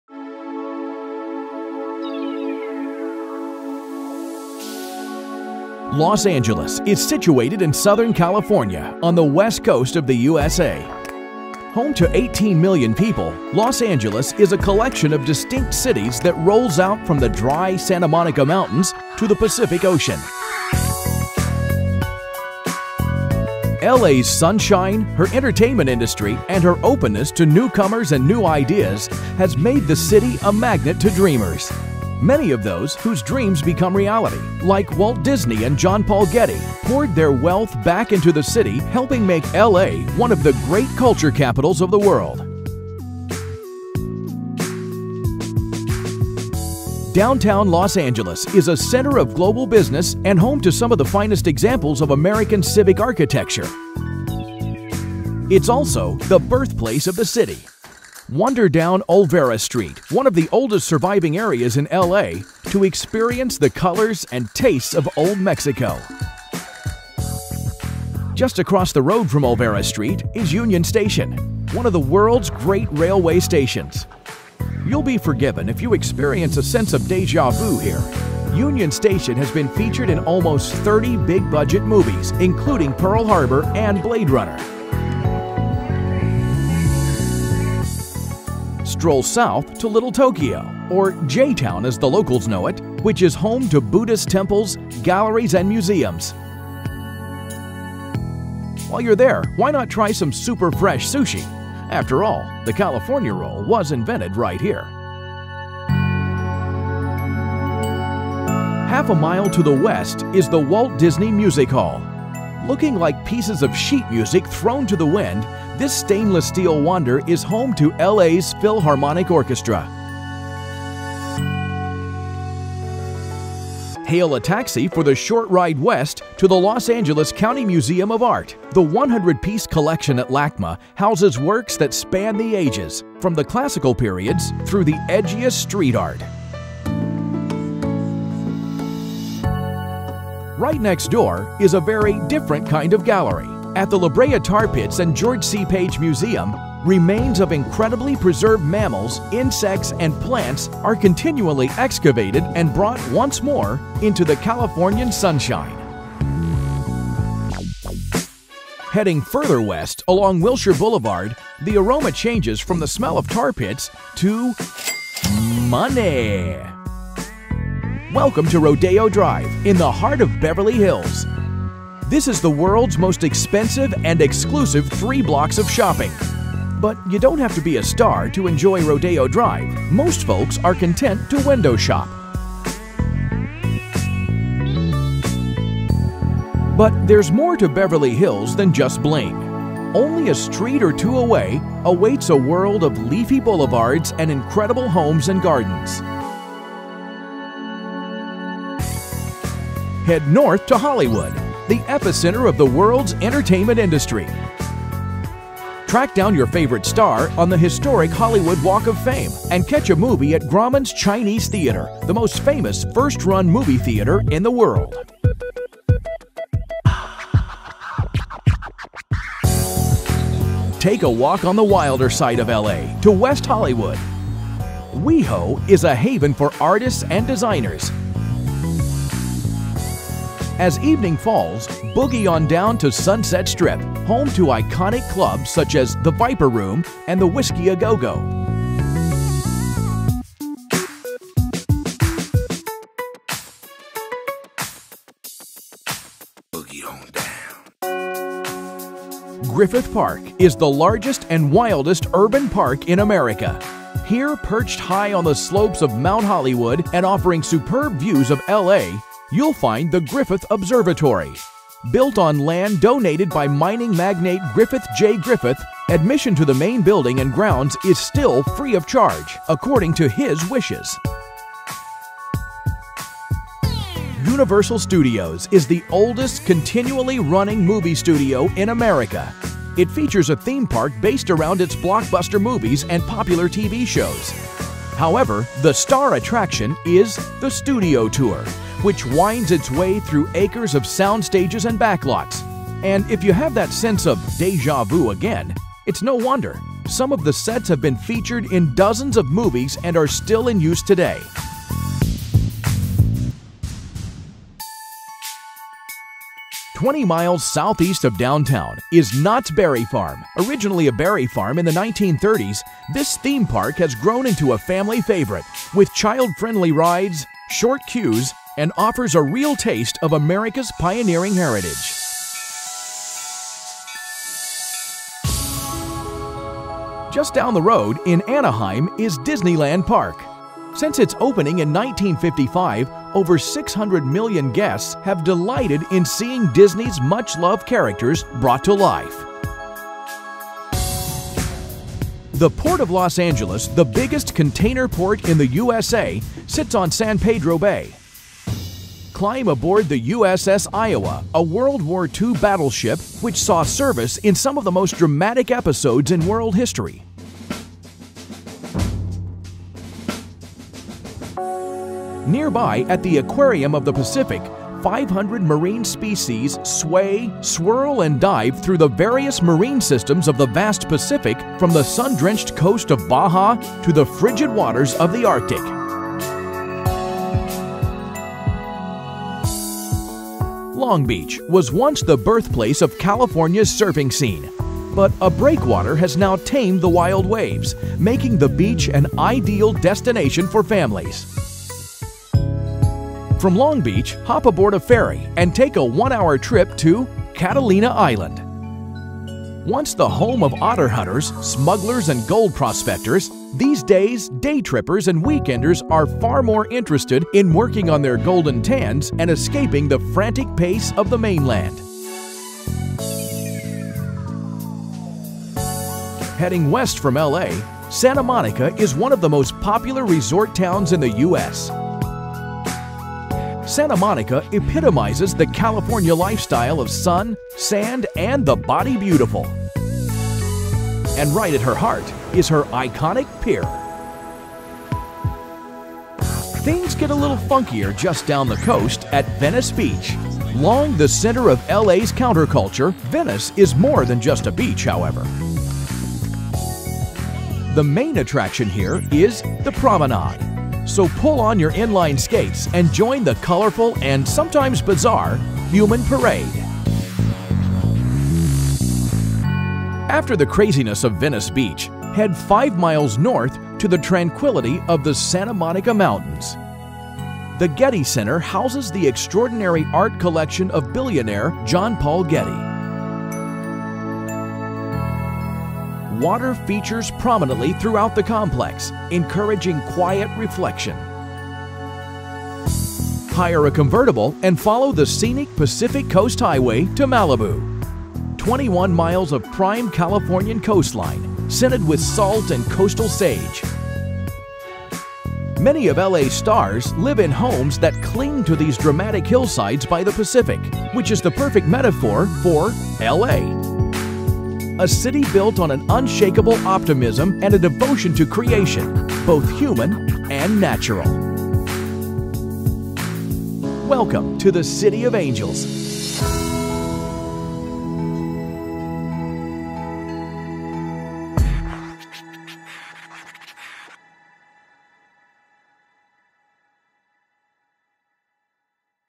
Los Angeles Vacation Travel Guide | Expedia
Check out our awesome footage of the sunny city of Los Angeles. You’ll want to pack your bags, grab your camera and join in on the fun here.

Welcome to LosAngeles – the “Capital of the Creative World.”

A vacation here begins and ends with the unique culture of the city and county. You will never experience another locale so devoted to the creation of art across all of its mediums. Visit the Getty Museum to see the sprawling collection of John Paul Getty. Take in the restored grandeur of Union Station – featured in numerous films – and then head for the world’s most famous zip code, 90210. Your trip to Beverly Hills will naturally include a stop at Rodeo Drive, where you can shop till you drop.

Los Angeles sightseeing is not complete without a stop at the Hollywood Walk of Fame, where you can see the handprints of your favorite stars. Don’t forget to get a picture with the legendary Hollywood sign in the background, or take a trip to one of the many famous theme parks around the city.

For now, we hope you enjoy watching this travel guide as much as we enjoyed making it.

0:00 - Los Angeles
0:46 - Downtown LA
0:57 - Olvera Street
1:07 - Union Station
1:28 - Little Tokyo
1:48 - Walt Disney Music Hall
2:03 - Los Angeles County Museum of Art
2:20 - La Brea Tar Pits and George C. Page Museum
2:38 - Rodeo Drive
3:18 - Hollywood
3:25 - Hollywood Walk of Fame
3:40 - West Hollywood
3:50 - Sunset Strip
4:08 - Griffith Park
4:13 - Griffith Observatory
4:33 - Universal Studios
5:05 - Knott’s Berry Farm
5:25 - Disneyland Park
5:40 - The Port of Los Angeles
5:48 - USS Iowa
6:01 - Aquarium of the Pacific
6:19 - Long Beach
6:32 - Catalina Island
6:55 - Santa Monica
7:15 - Venice Beach
7:39 - Santa Monica Mountains
7:47 - The Getty Center
8:02 - Malibu